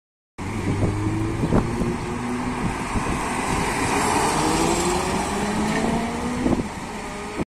Автобус ПАЗ-320435-04 "Vector Next" (NS) № С 493 ТА 62 №94 маршрутом №77
Автобус ПАЗ-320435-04 "Vector Next" (NS) № С 493 ТА 62 №94 (ИП Серёжин С.И.) маршрутом №77 "Пос. Мехзавода - Пос. Карцево" следует по ул. Чкалова, г. Рязань, Рязанская область. 14:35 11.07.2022 г.